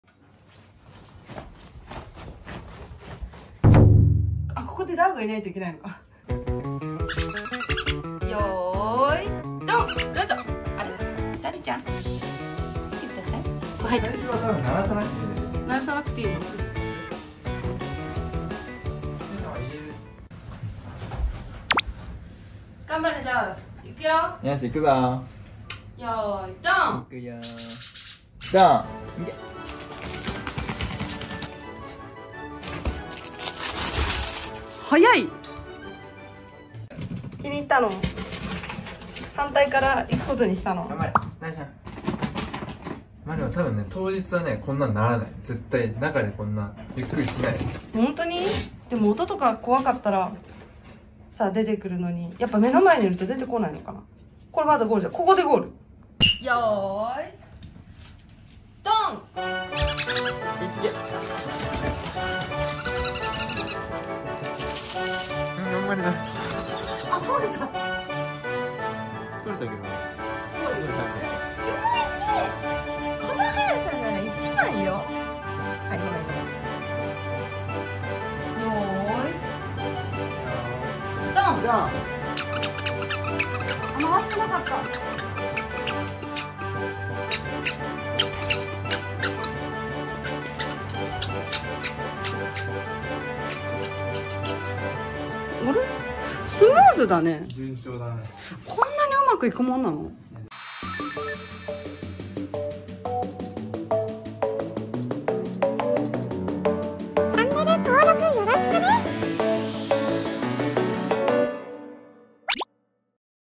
チューブレースの練習したよ！絶好調！フェレット
フェレットのダウ(♂)です。
ついに明日！！11/12(日)、チューブレースです！
ダウしゃん初出場です🐼✨
ということでこの間練習したのでその様子を・・！
慣れている家の中だからかものすごく順調。(笑)
会場はお外なのでどうなるか楽しみですっ(笑)(笑)

【音楽】
～騒音のない世界～